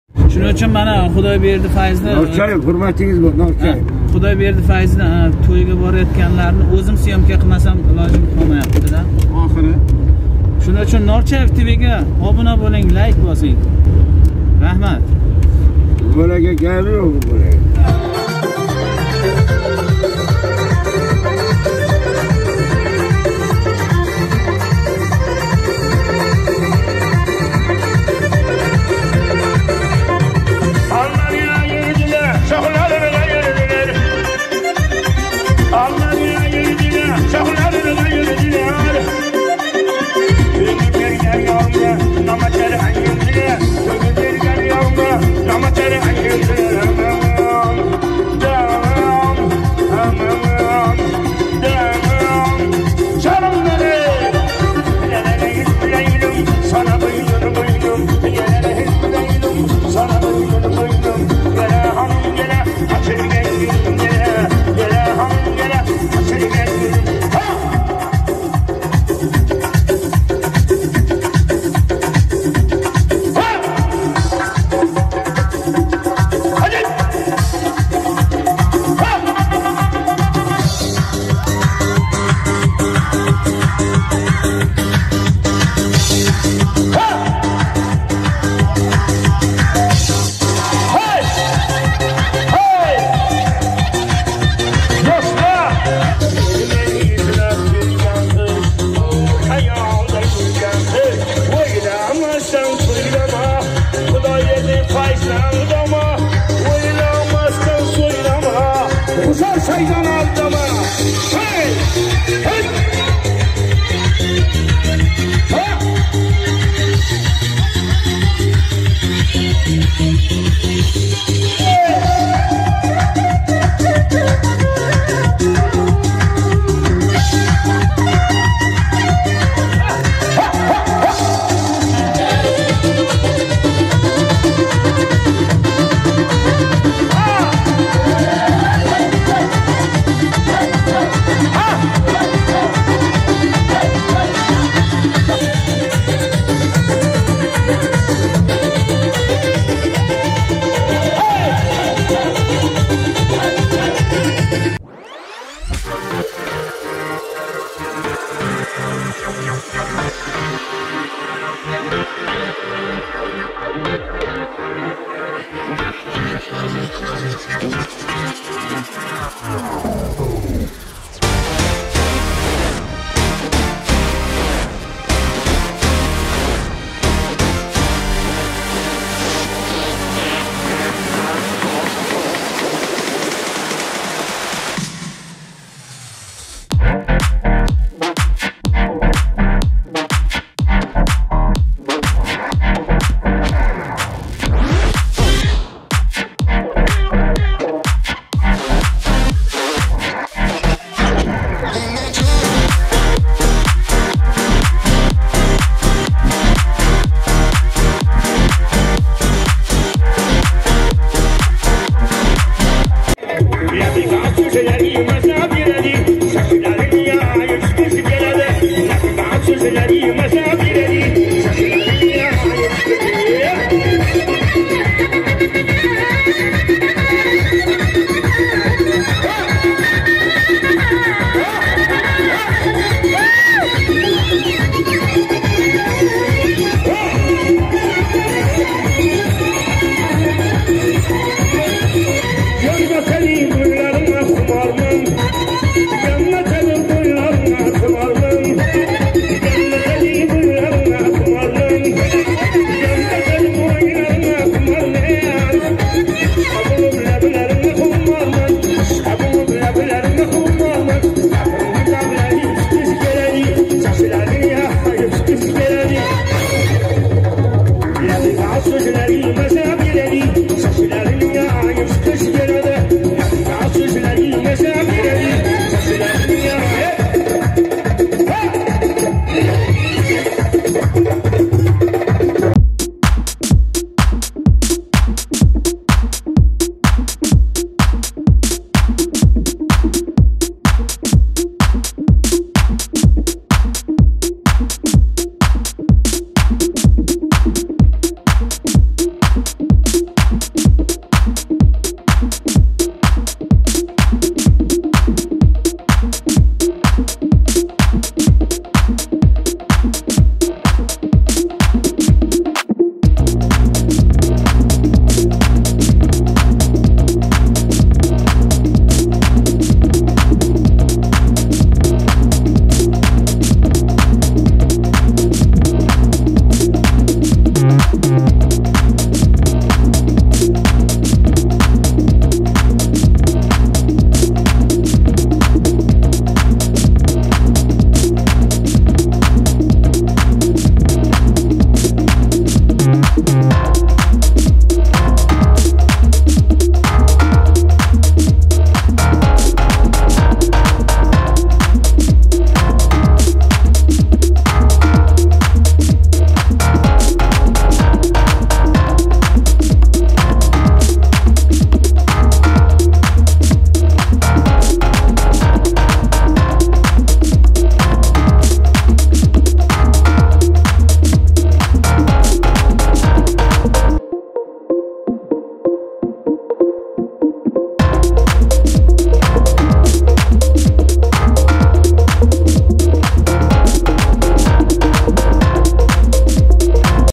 Гузор туйлари 6 сон | Xudoyberdi Fayz | Norchayev Tv | Гузор янгиликлари G‘uzor tumani
‐--‐ NorchayevTv
Assalomu alaykum aziz yurtdoshlar layk bosib bizni qo‘llab quvvatlashni va obuna bo‘lib qo‘yishingizni iltimos qilib qolgan bo‘lar edim!
‐--‐ NorchayevTv

Qashqadaryo viloyati G‘uzor tumani Xudoyberdi Fayz to‘y videolari.

‐--‐ NorchayevTv